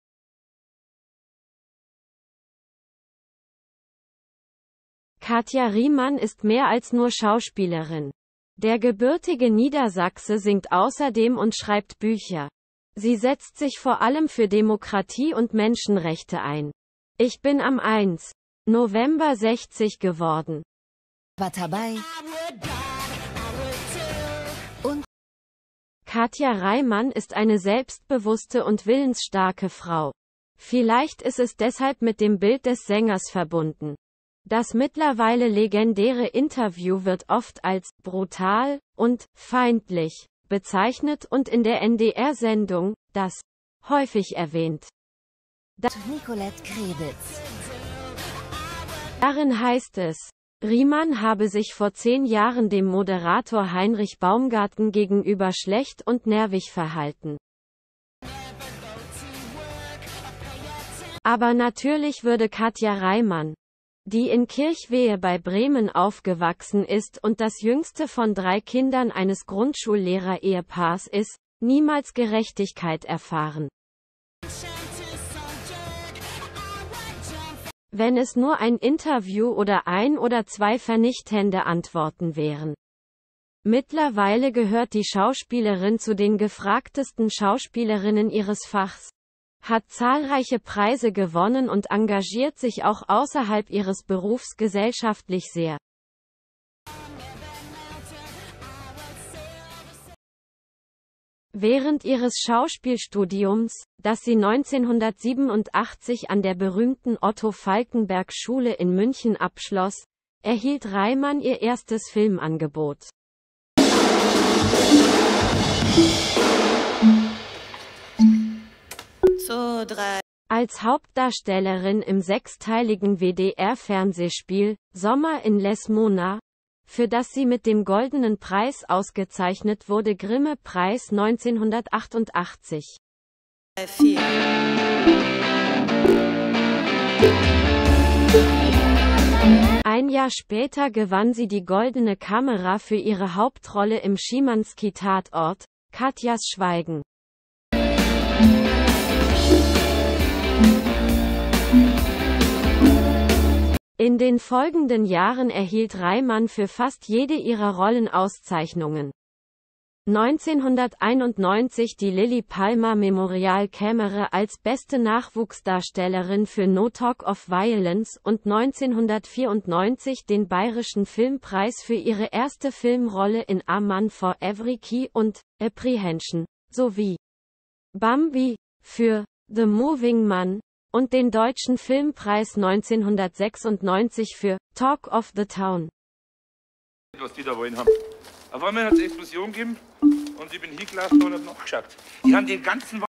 (REAKTION) Der NDR ehrt Katja Riemann, weil sie mehr als nur Schauspielerin ist.
Katja Riemann ist mehr als nur eine Schauspielerin. Die gebürtige Niedersächsin singt auch und schreibt Bücher. Vor allem aber engagiert sie sich für Demokratie und Menschenrechte. Am 1. November wurde sie 60 Jahre alt.
Katja Riemann ist eine selbstbewusste und willensstarke Frau. Wahrscheinlich haftet ihr auch deshalb das Image einer Diva an. Sie sei "biestig" und "zickenhaft", heißt es oft, und meist wird auf ein inzwischen legendäres Interview in der NDR Sendung "DAS!" verwiesen, in dem sich Riemann vor zehn Jahren gegenüber Moderator Hinnerk Baumgarten übellaunig und nörgelig verhielt.
Doch natürlich wird man Katja Riemann, die in Kirchweyhe bei Bremen als jüngstes von drei Kindern eines Grundschullehrer-Paares aufwuchs, überhaupt nicht gerecht, wenn sie auf dieses eine Interview oder auf die eine oder andere schnippische Gegenrede reduziert wird. Denn die Schauspielerin zählt inzwischen zu den gefragtesten ihrer Zunft, hat jede Menge Preise eingeheimst und engagiert sich abseits ihres Berufs in hohem Maße sozial.
Schon während ihres Schauspiel-Studiums, das sie 1987 an der renommierten Otto-Falkenberg-Schule in München abschloss, erhielt Riemann ihr erstes Filmangebot: Als Hauptdarstellerin im sechsteiligen WDR-Fernsehspiel "Sommer in Lesmona" bekam sie 1988 den Grimme-Preis in Gold, ein Jahr später gab es für die Titelrolle im "Schimanski"-Tatort "Katjas Schweigen" die "Goldene Kamera